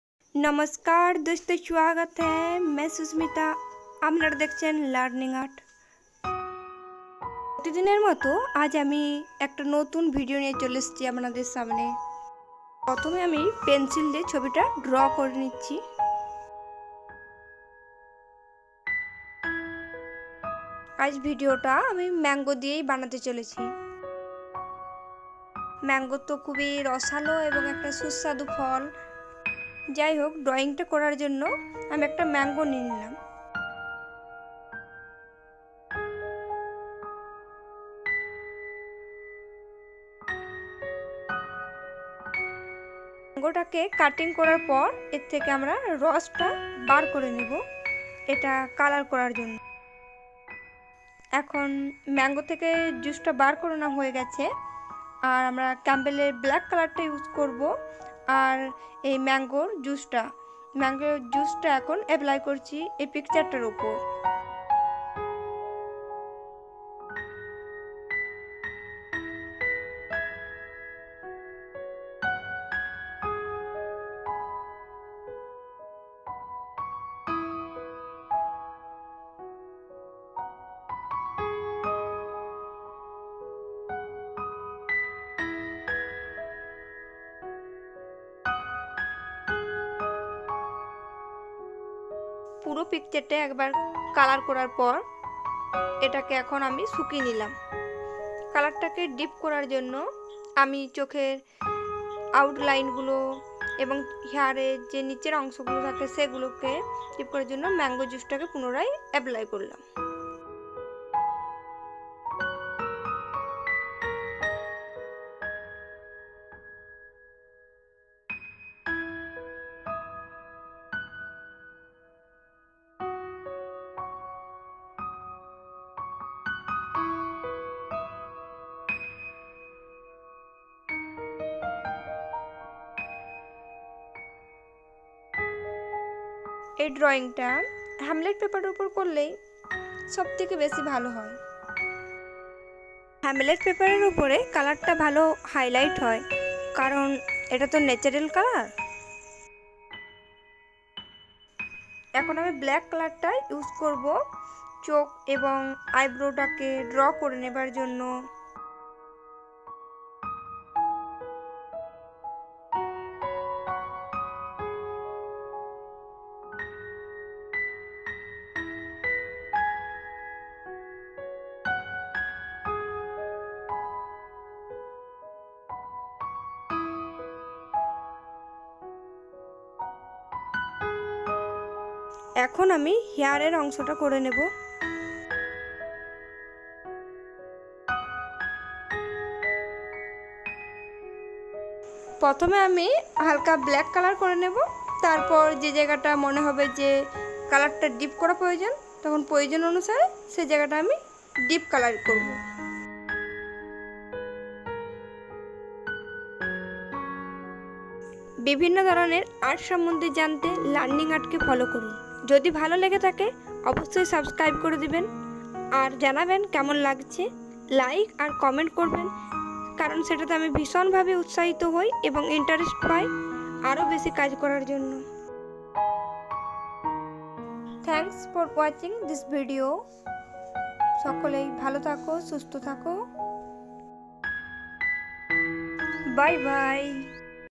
Drawing with Mango 🥭🥭 Natural colour painting || Girl drawing with Mango🤠
In this video i am painting girl with mango extract.

I hope u enjoy this drawing video. This video is a painting tutorial with natural colours, not only safe to use it's cheaper on your wallet too. You need not depend on stores every time you need something.

Chances are more that it might consume paint and here you don't have to stress about it if they do. I would suggest u to use these non-toxic material colour and i am sure u will love it.

Drawing material -
                             (1)pencil
                             (2)hamlet paper
                             (3) mango
                             (4)camel black colour
                             (5) brass

      Drawing a land scape natural colour with camel acrylic colour.